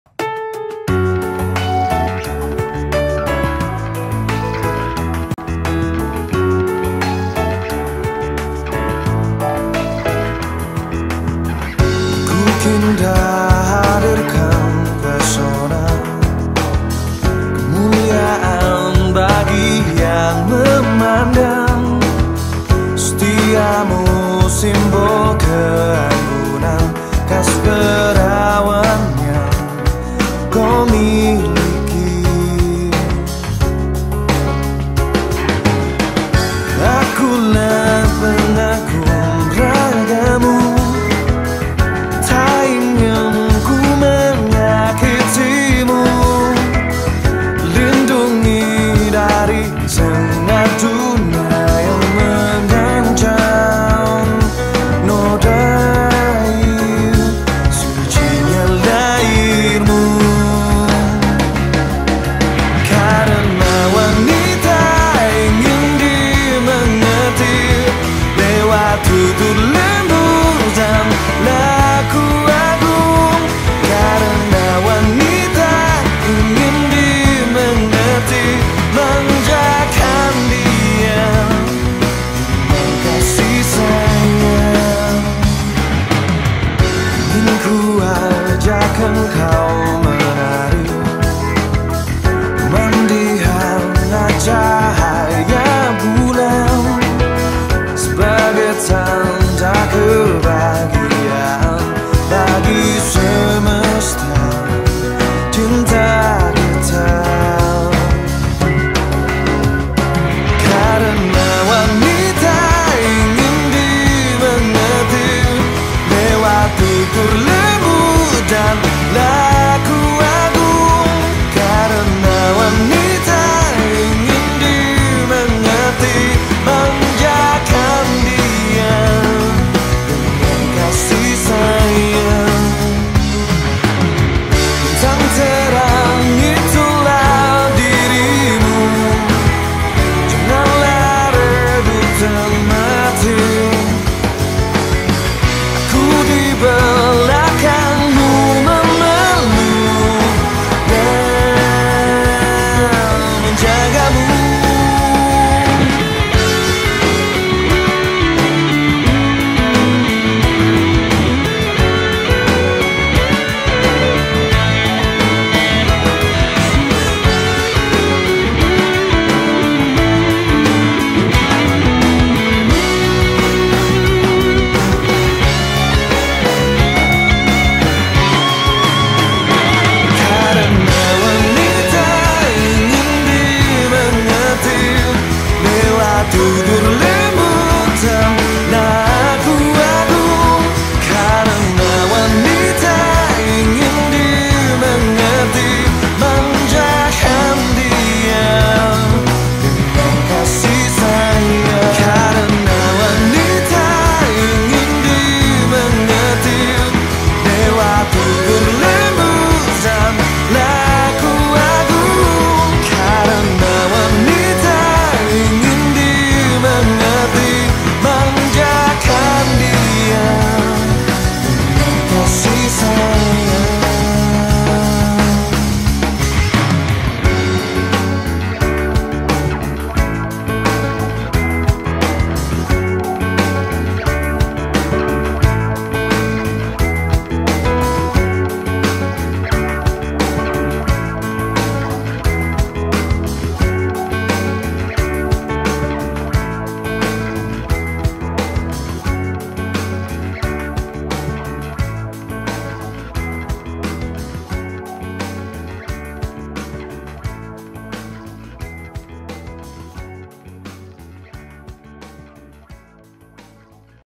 Donnie Sibarani & Krishna Balagita ex Ada Band konser
Konser musik bertajuk Gentleman Story Tgl 23 november 2019 di colorspub surabaya

Wassalamulaikum salam damai semesta